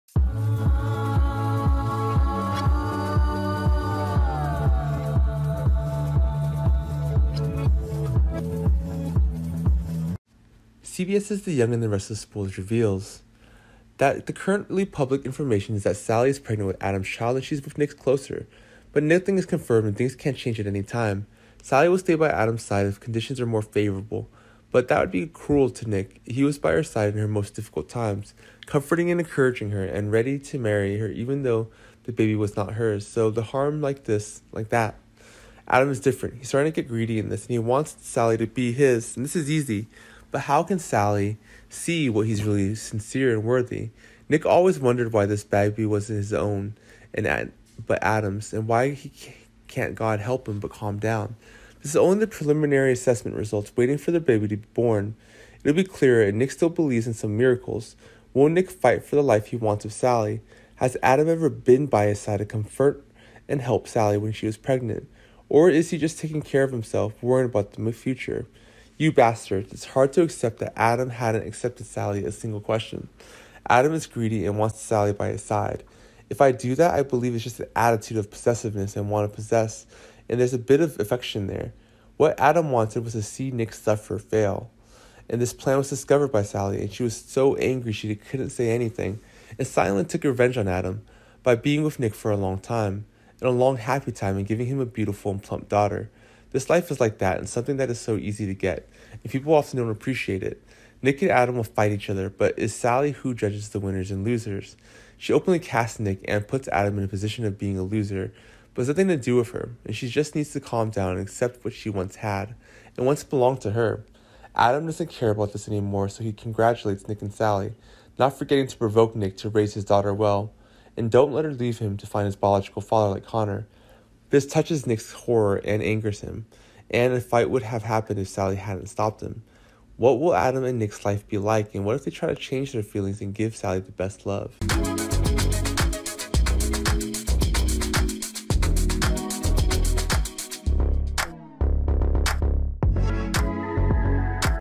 Young And the Restless Spoilers Adam pretends to love Sally to defeat Nick - revenge for that baby
Young And the Restless Spoilers Y&R Adam pretends to love Sally to defeat Nick - revenge for that baby.